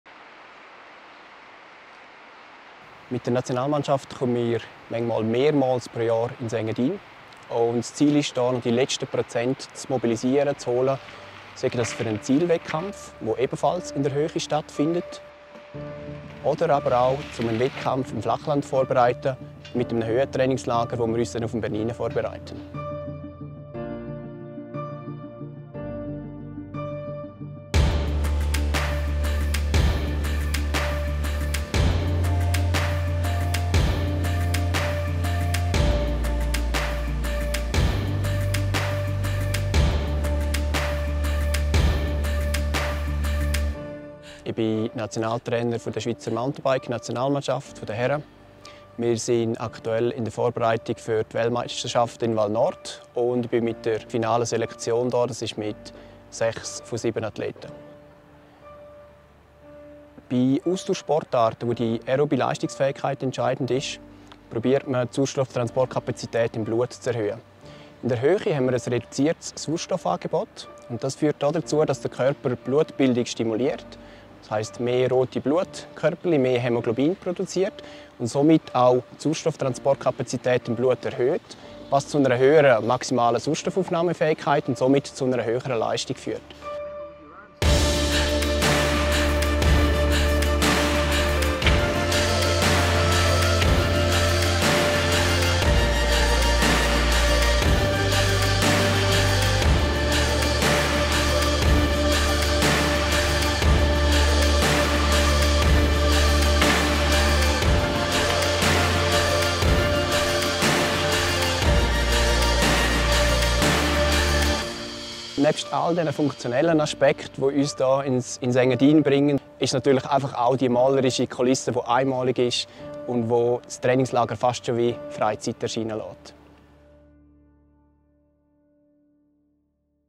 Engadin Stories - Swiss Cycling
Swiss Cycling nutzt das Engadin seit Jahren als festen Bestandteil ihrer Saisonvorbereitung. Hier feilen die Athleten und Athletinnen an ihrer Form, testen neue Strategien und optimieren ihre Technik. Dank der hochmodernen Infrastruktur und der professionellen Unterstützung vor Ort kann sich das Team voll und ganz auf ihr Training konzentrieren.

Folge uns!